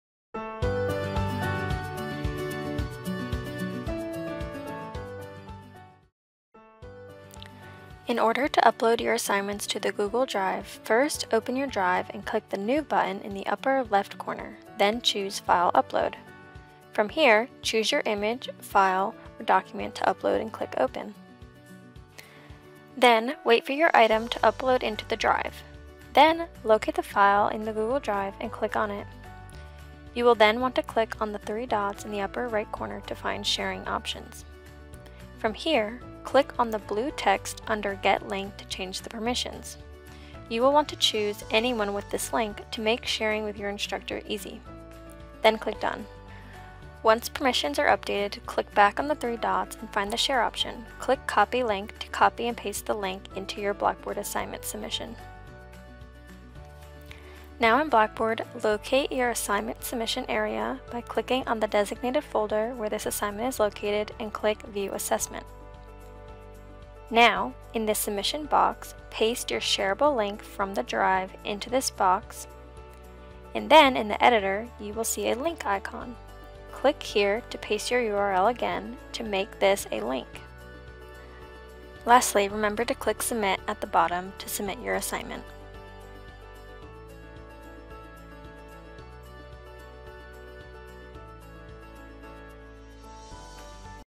How to Upload Assignments to Google Drive and Submit in Blackboard
A brief student tutorial for uploading assignments to the Google Drive and submitting the shareable link to the Blackboard submission area.